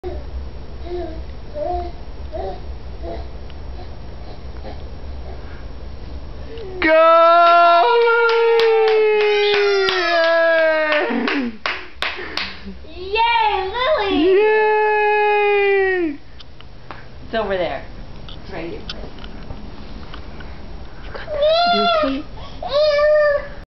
Lily walks for the first time!
She just had to keep her eye on the ball!